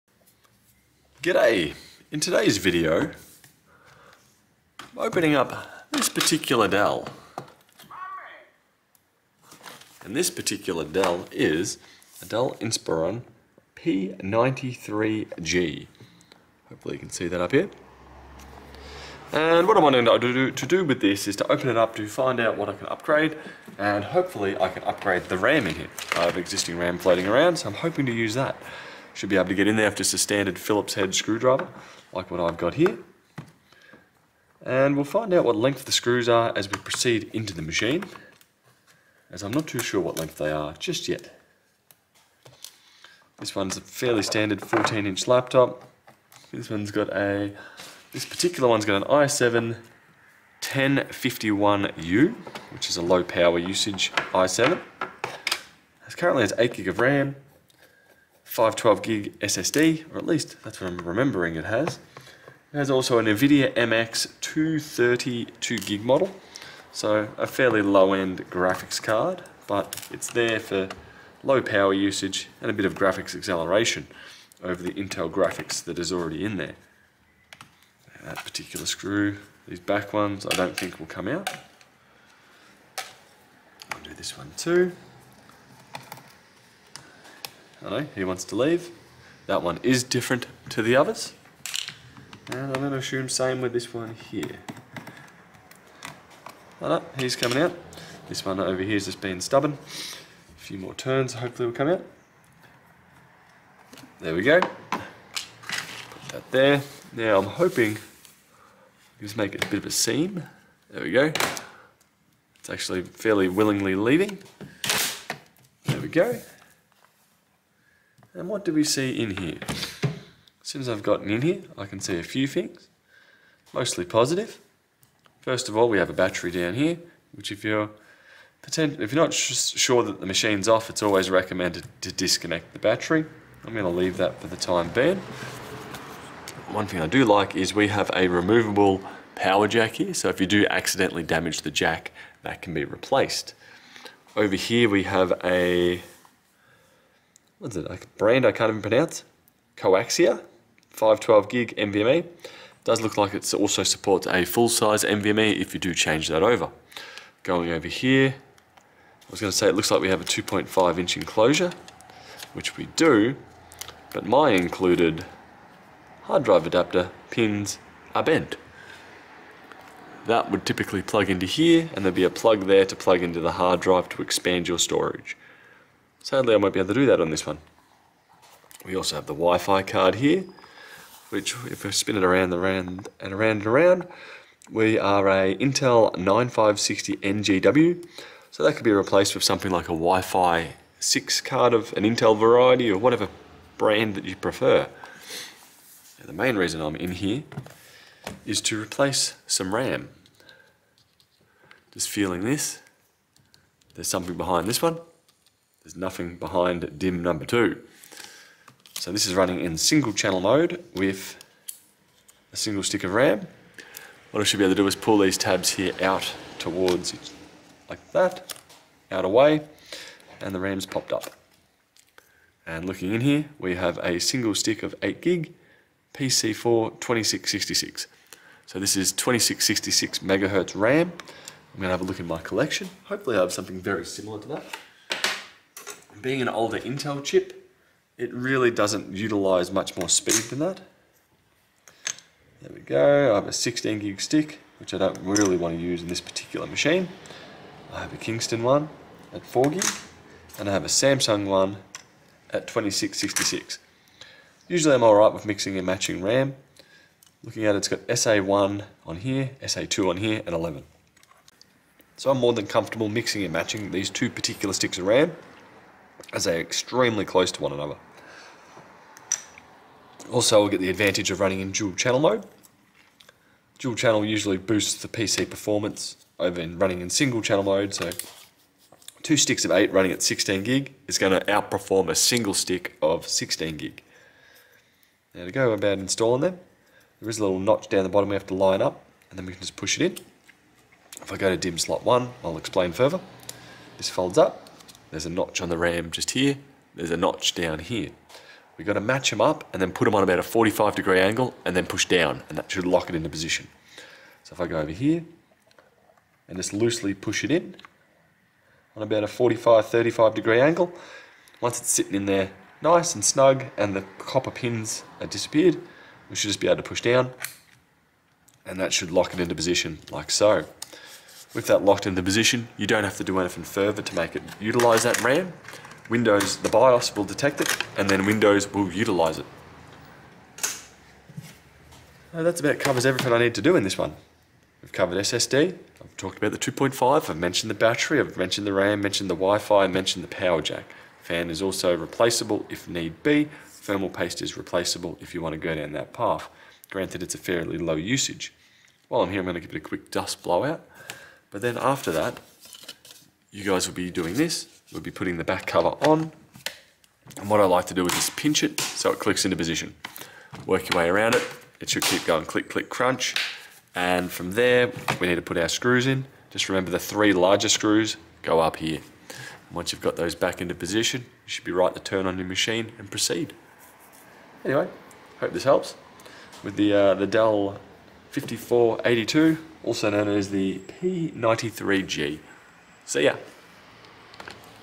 Dell Inspiron 15 P93G Upgrade Options / Ram upgrade
In this video I'm upgrading the ram in a Dell Inspiron 15 P93G
While im in there i have a look and see what else can be upgraded